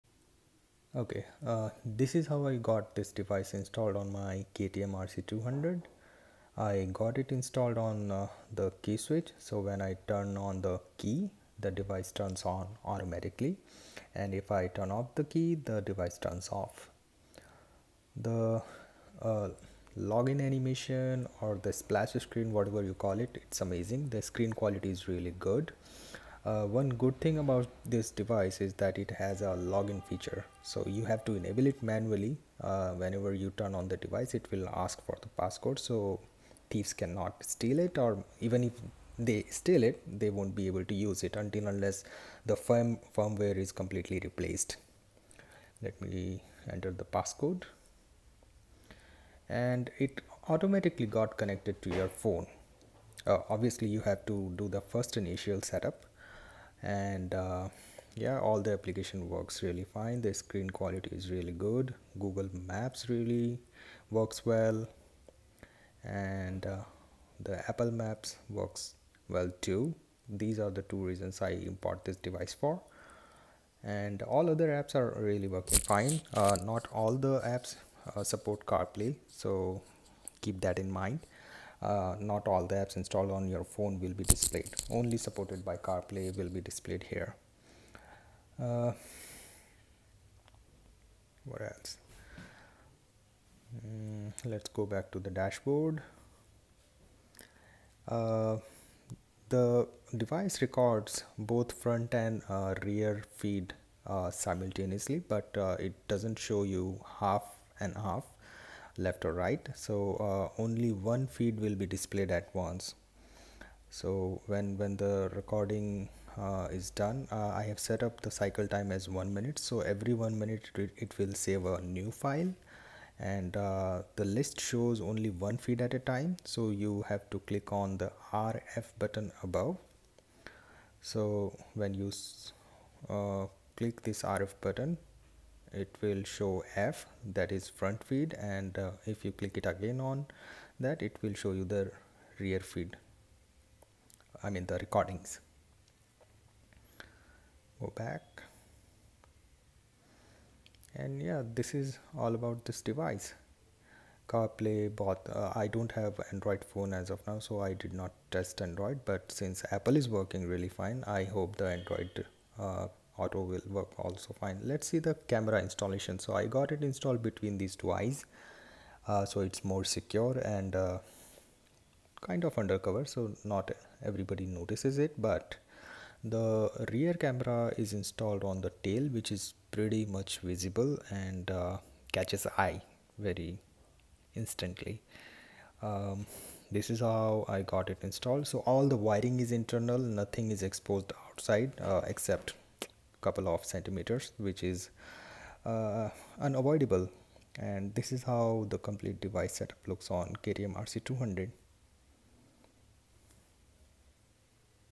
Apple CarPlay / Android Auto On KTM RC 200 (Cleanest Installation On YouTube)
I bought it for 10K in Amazon Great Indian Sale (Now its 12K). I didn’t see any reviews on this seller however, it was around 300rs cheaper than other seller with 7 days Returnable Option while the other sellers had only replace option. Took the chance and placed the order.  I was skeptical where it will fit on my KTM RC 200 as it has a clip-on handlebar not the pipe like handlebar (for which the included mounts are for). I have a 3d printer so i thought i’ll figure out something and print a custom mount if required.

Apart from the device and the mount for regular handlebar there is small mount which i think is to install on the screws. I am very peculiar how clean and nice the things look so i didn’t want to install it on side. The battery cover is very thin plastic so i was sceptical it will break after sometimes. I had a GoPro mount already installed on the battery cover so decided to use that as stronger base.

Went to the Custom Bike Builder in my City (Kunwar Customs In Jaipur). They did the wiring job very neatly and drilled the hole on the GoPro mount as i suggested and installed on the battery cover which now feels very strong. I can unplug the device and  take out when i park it anywhere or even at home.

The entire project went really well and i am happy with how it looks on the bike. The device supports 64GB SD card and the entire installation and usage experience so far is excellent. Few people have complained on other seller page that it keeps rebooting itself after some time (maybe overheating? I don’t know! ). I have not tested it yet for long rides. I will test it later.

Do check the Price for other sellers for cheaper options. Try searching Motorcycle Carplay.

On AliExpress its available in 4-5K but thanks to Governments China Ban. Everything is double the price in India even when we import more than 80-90% electronics from China.